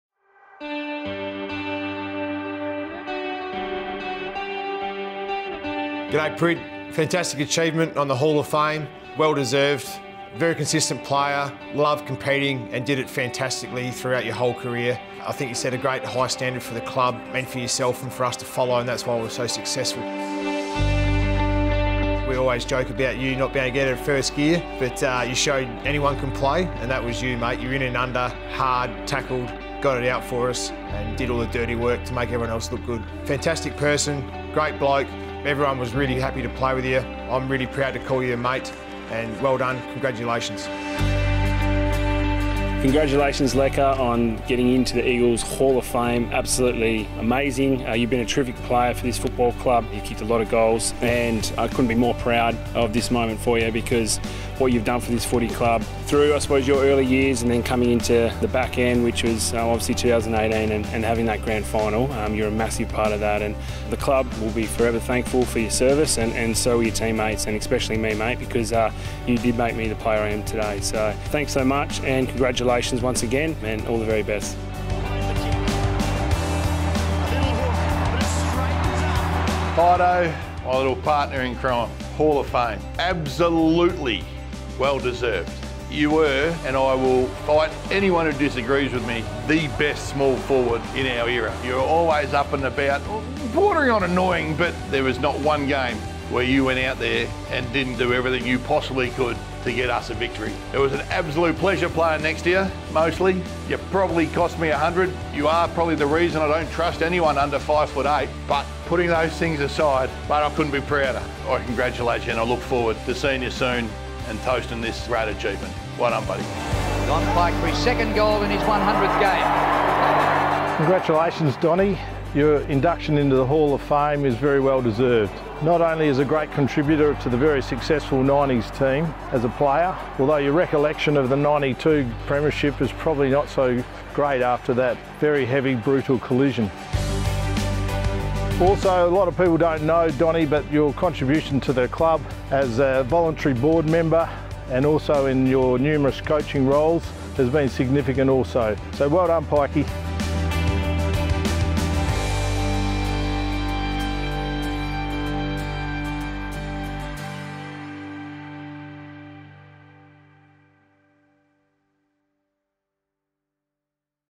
Hall of Fame testimonials
Former teammates of this year's Hall of Fame inductees share their congratulations